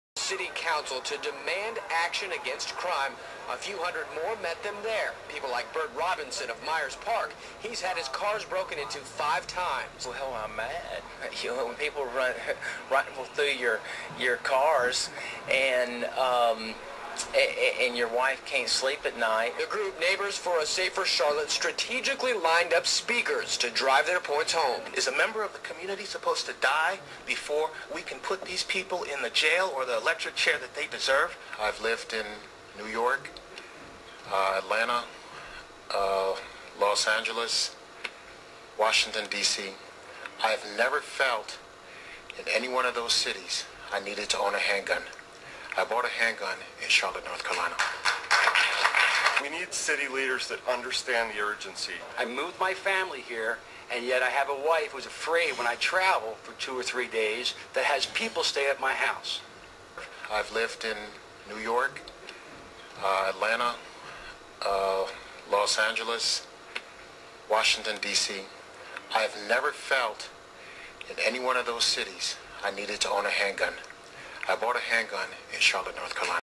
Pat McCrory's Crime Rate
Charlotte NC has a crime rate about double that of Raleigh NC.  Pat McCrory wants to bring his kind of "success" to Raleigh.  We don't need it. We don't need a higher crime rate.